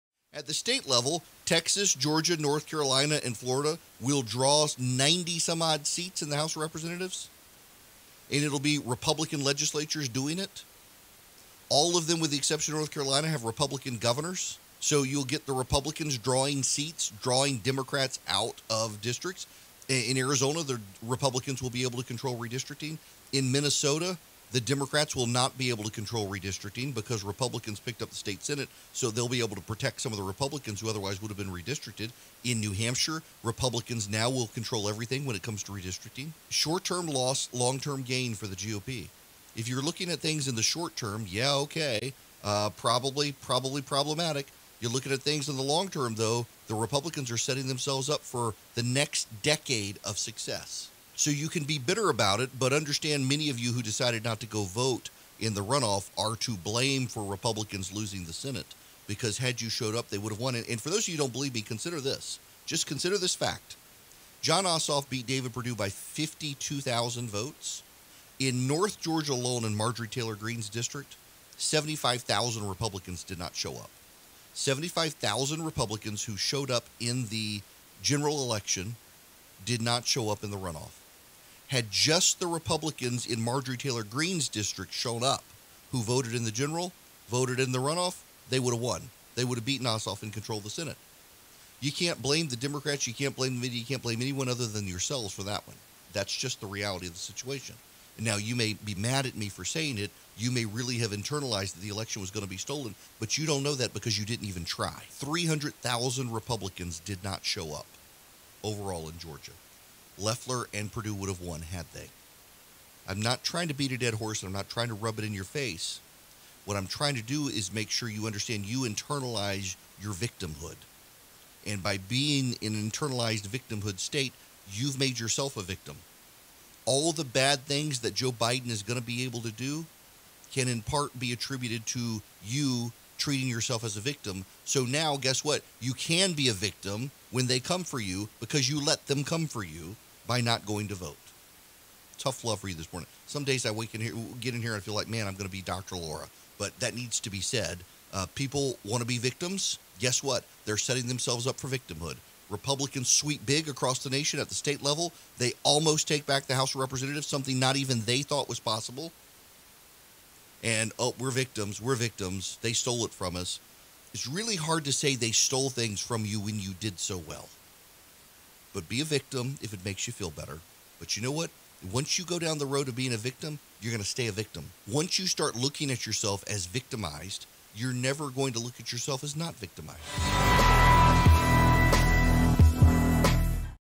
Some tough love for Republicans: Short term disaster but long term success?
Republicans picked up more seats in 2020 than anyone predicted. However, Republicans need to hear this tough love.